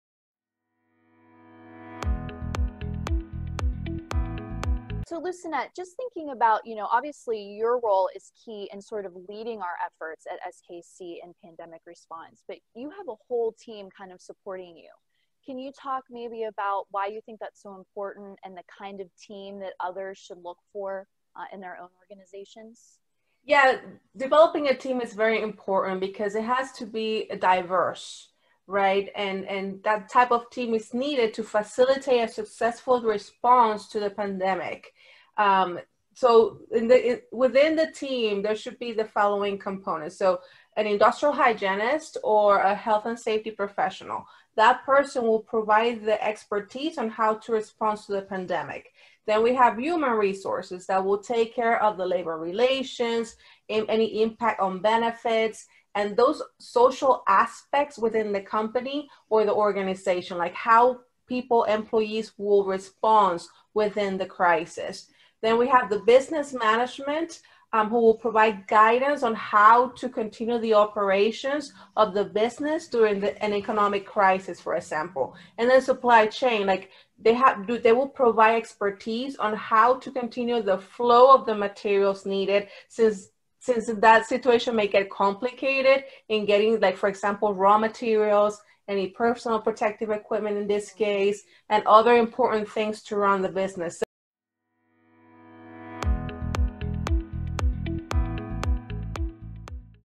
COVID-19 Q&A: Developing a Pandemic Response Team
SKC's Lucinette Alvarado, Corporate CIH, answers a follow-up question from our "COVID-19 and Best Practices for Re-Opening" webinar: What should organizations look for when developing a pandemic response team?